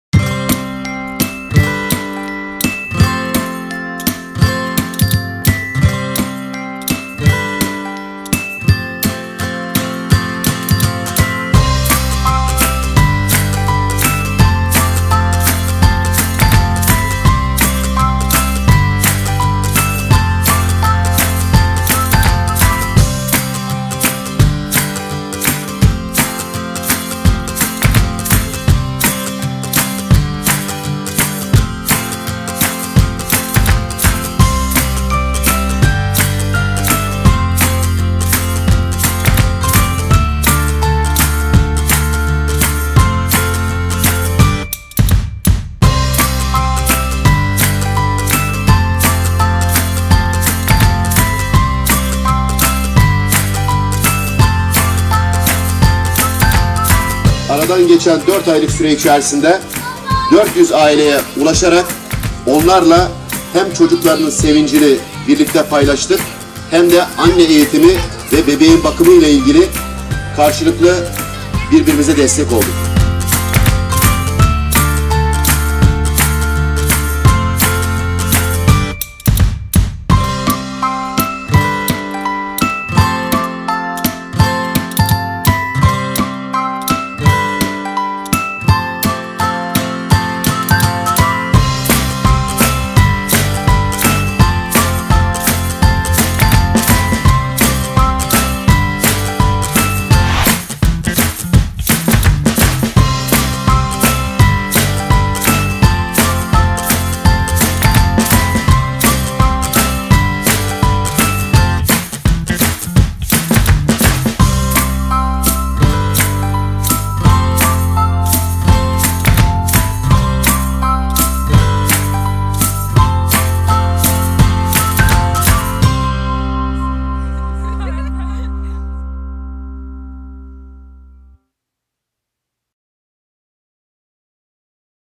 Milas Belediyesi Hoşgeldin Bebek Etkinliği
Hoşgeldin Bebek projesi kapsamında bebeklerimizle ilk bayram heyecanını yaşadık